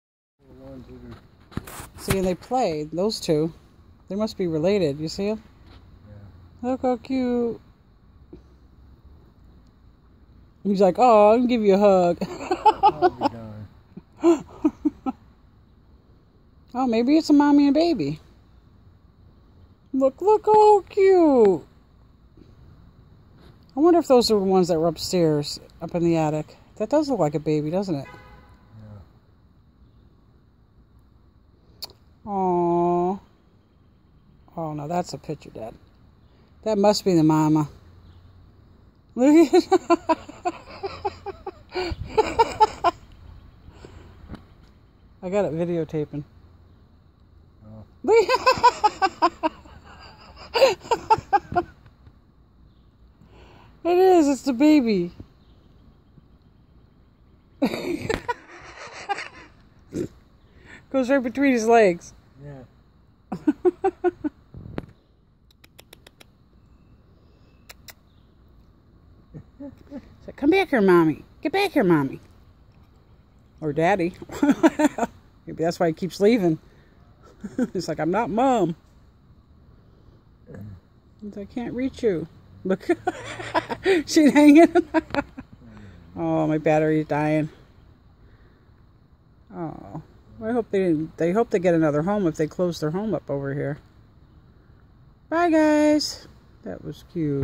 GOT MILK? Homeless, mama and me...
Theses 2 homeless squirrels were coming closer than any other,  I threw them peanuts but they didn't want to take part,  then the I thought they where male and female but the sweetest thing happened